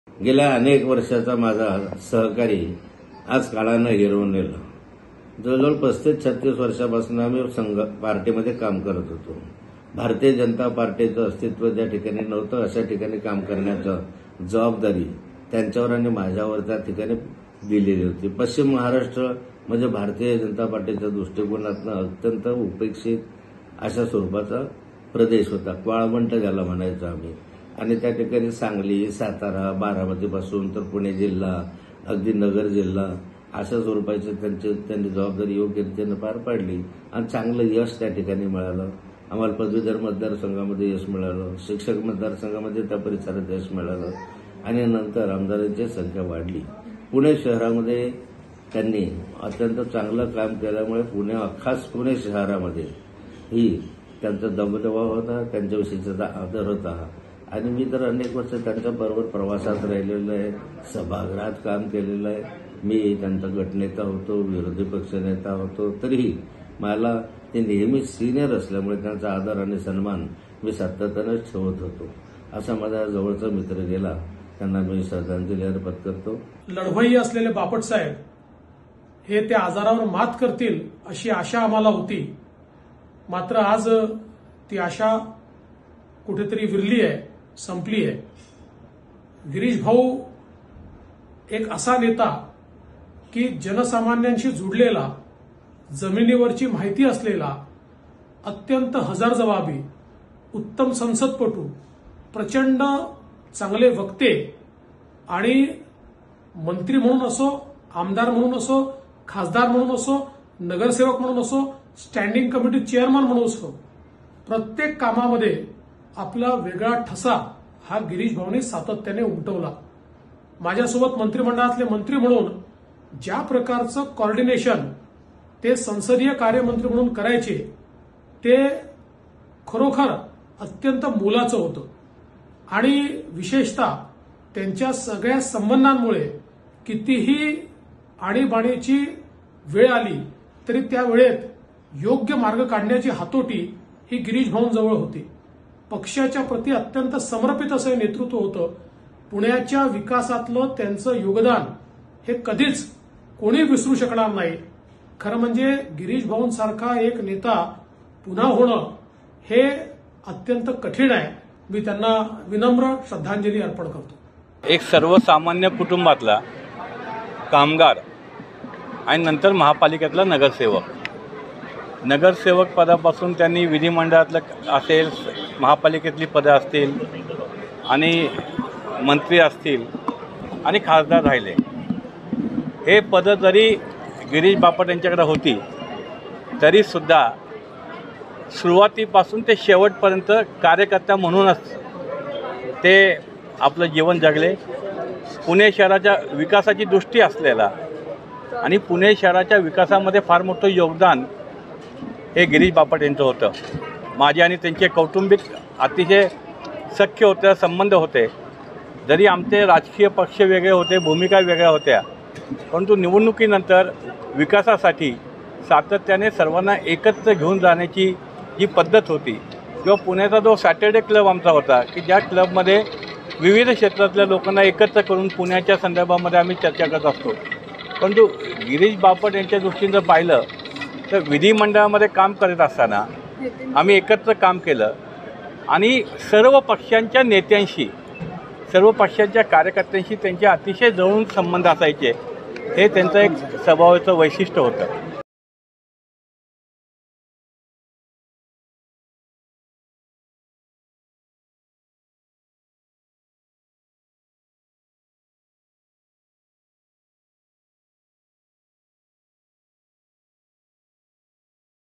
Girish Bapat Death | बापटांच्या निधनानंतर लोकप्रतिनिधी भावूक | LetsUpp Marathi
भाजपचे ज्येष्ठ नेते गिरीश बापट यांच्या निधनानंतर प्रतिक्रिया देताना अनेकांना भावना अनावर झाल्याचे दिसून आले.